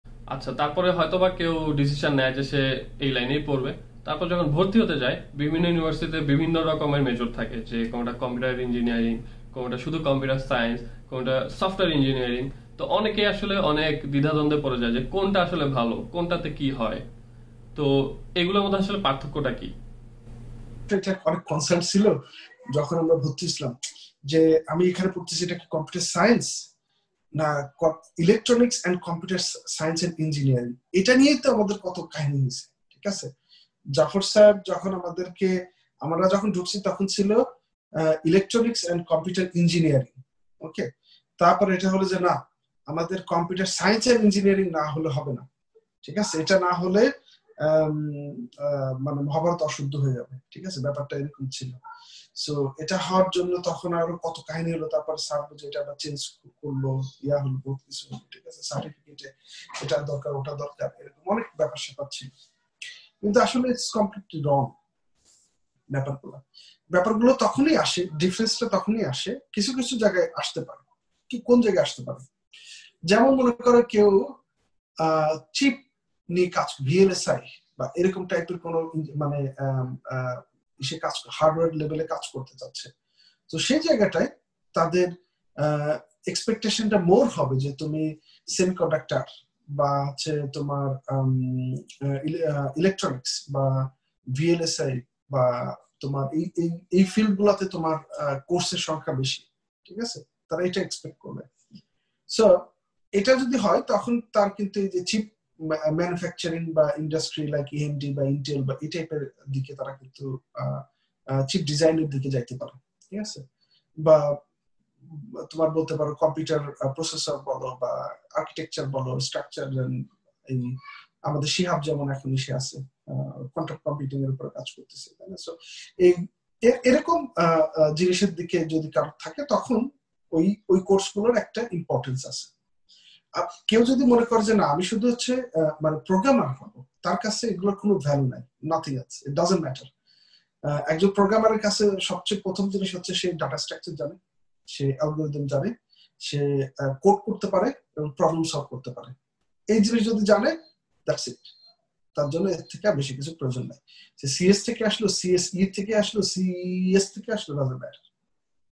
CSE/CS/SWE/EECS/ISCE/IT কোনটা ভালো?
আন্ডারগ্র্যাডে বিভিন্ন ইউনিভার্সিটি অনেক নামে কম্পিউটার বিজ্ঞান রিলেটেড মেজর অফার করে। কোনটা আসলে ভালো?